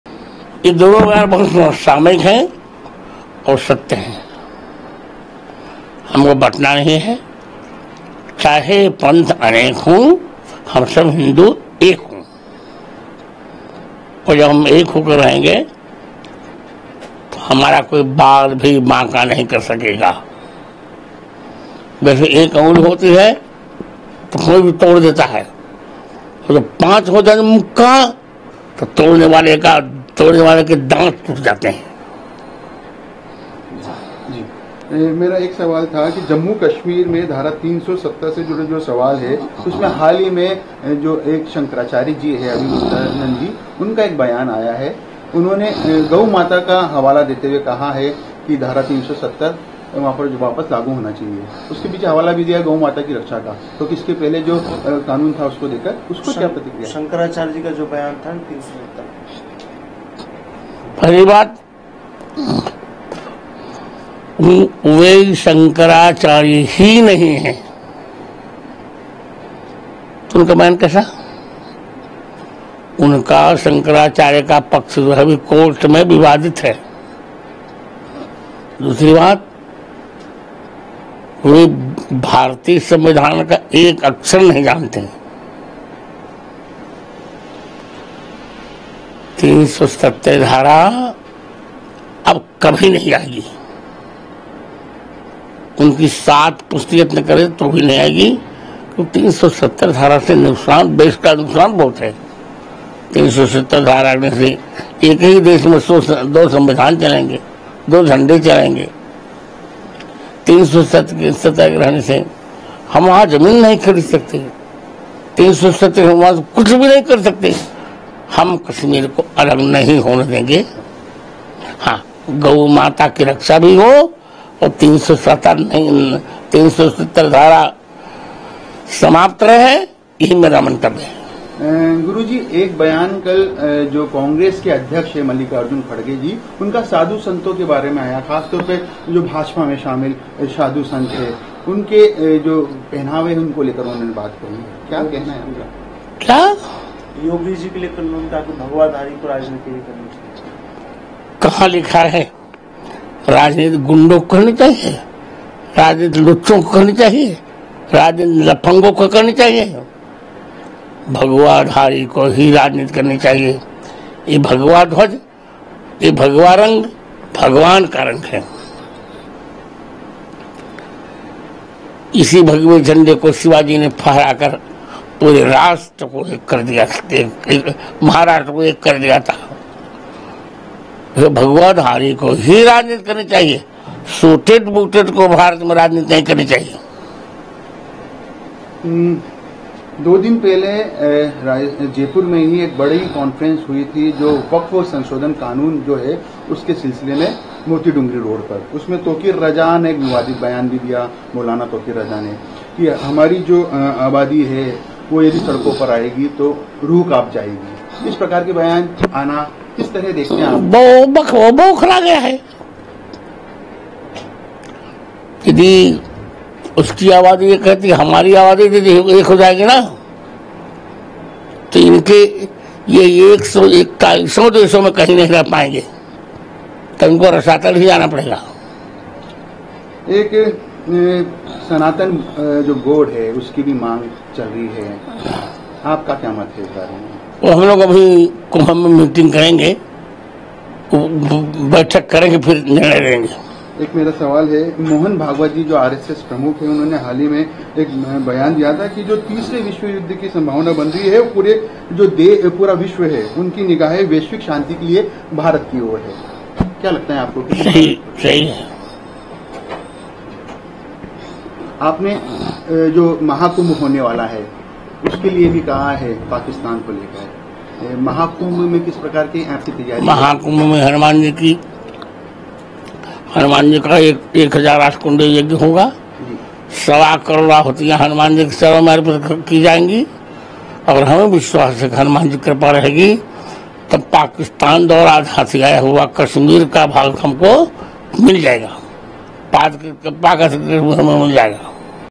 Jaipur में Rambhadracharya ने कटेंगे तो बटेंगे वाले नारे पर दिया बयान, बोले "रसातल में चले जाएंगे"
Jaipur में Rambhadracharya ने कटेंगे तो बटेंगे वाले नारे पर दिया बयान, बोले "रसातल में चले जाएंगे" TV9 Rajasthan